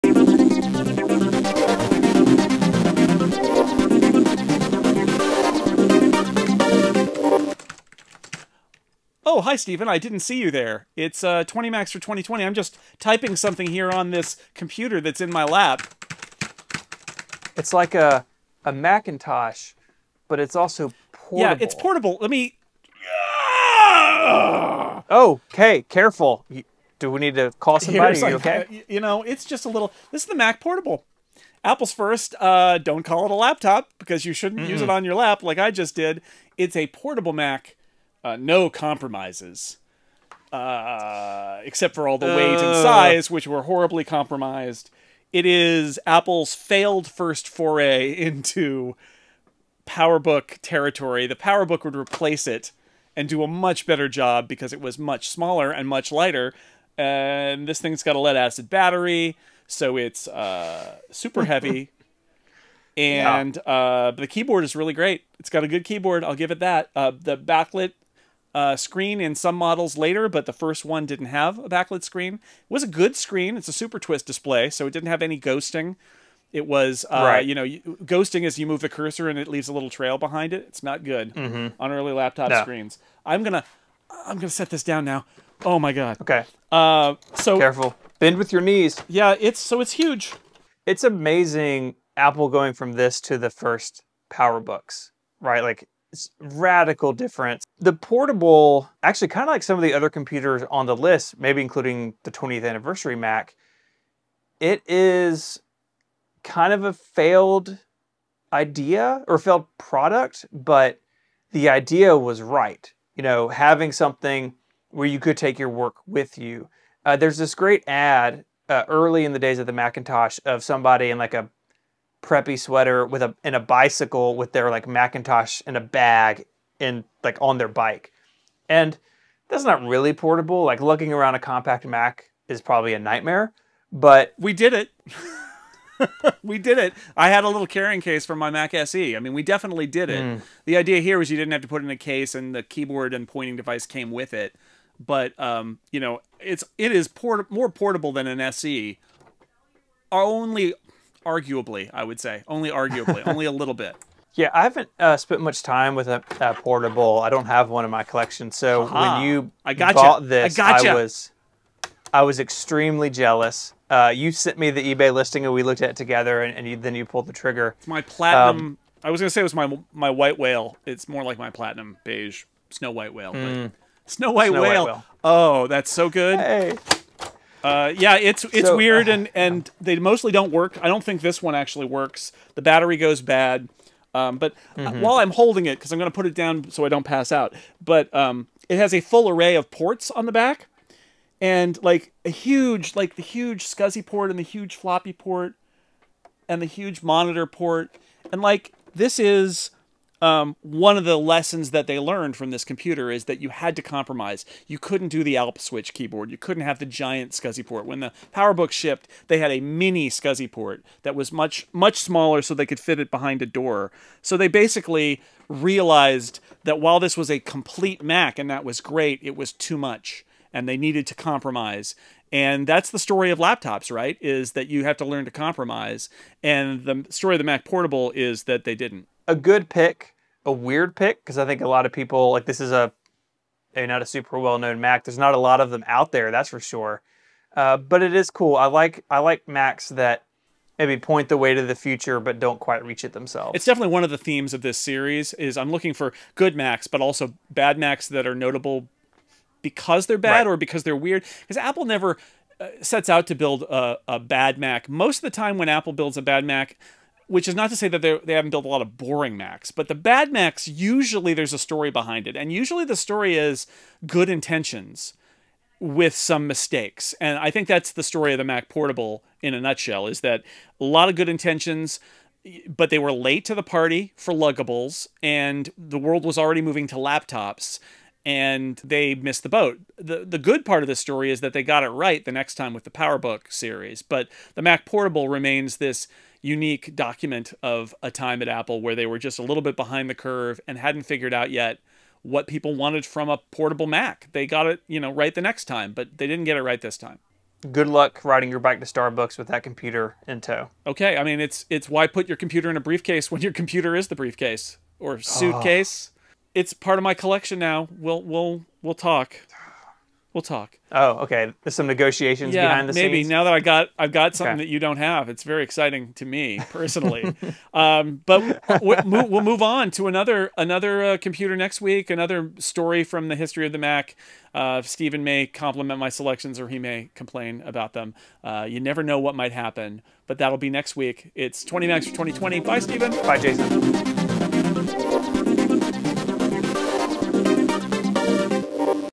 Mac Portable #11 (20 Macs for 2020)
Before the PowerBook came along, Apple's idea of a portable Mac was ... a bit different.